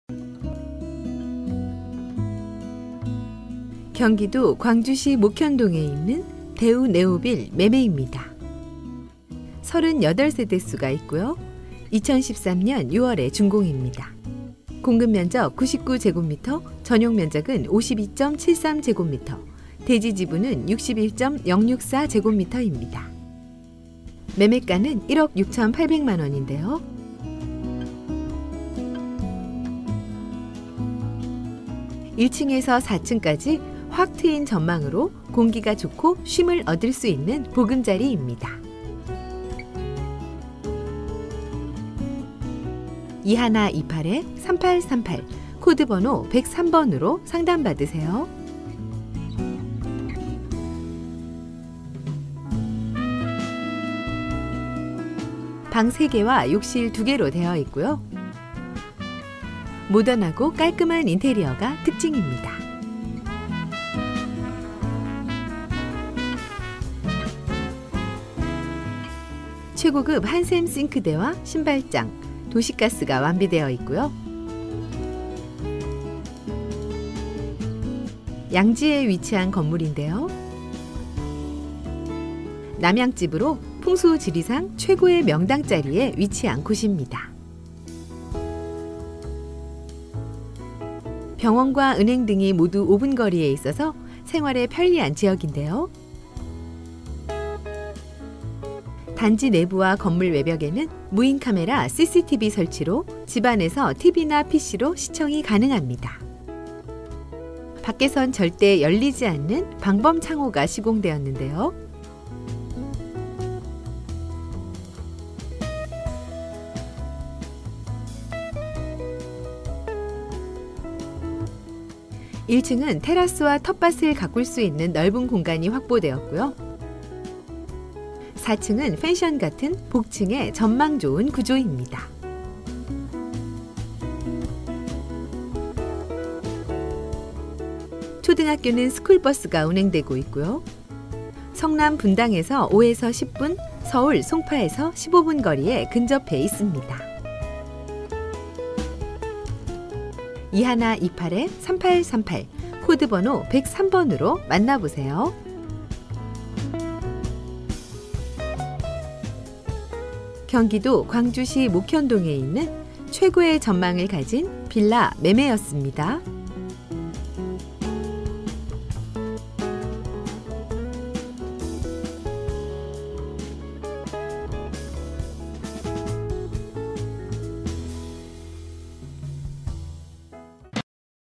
[거래 완료]목현동 대우네오빌
알토마토

방3개 화장실 2개의 확트인 구조의 1층은 테라스및 텃밭을 가꿀수있는넓은 공간확보...장독및 화분들을 놓기넘좋아요
4층은 펜션같은 복층의 전망좋은 주변환경으로 늘 맑은 공기와 전원생활을 즐기수있어 넘좋습니다.
최고급 한샘씽크대및 신발장, 도시가스완비.양지에위치한건물에 남향집. 지관이 인정한 풍수지리...